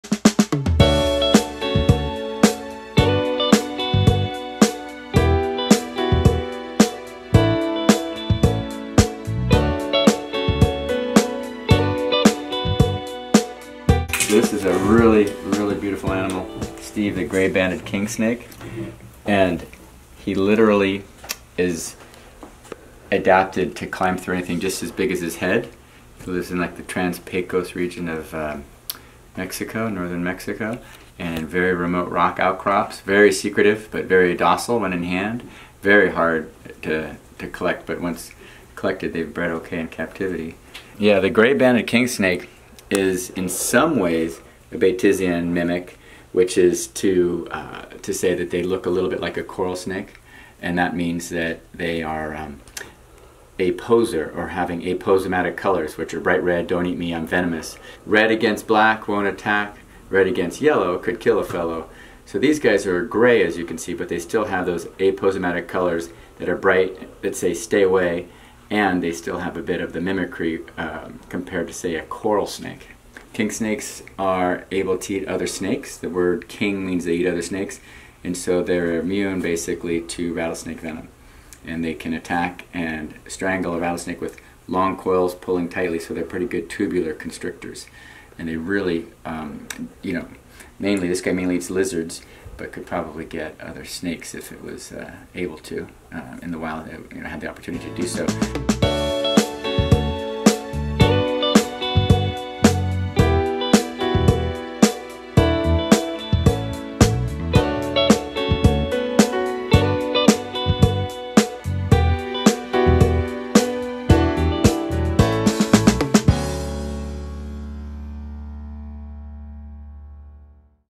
Gray Banded Kingsnake - Tree Frog Treks
Welcome to Tree Frog Treks and to one of their Animal Ambassadors named Steve, a Gray Banded Kingsnake. Chris Giorgi, Mr. Science, will tell you a little about Steve and some of the cool things about Gray Banded Kingsnakes.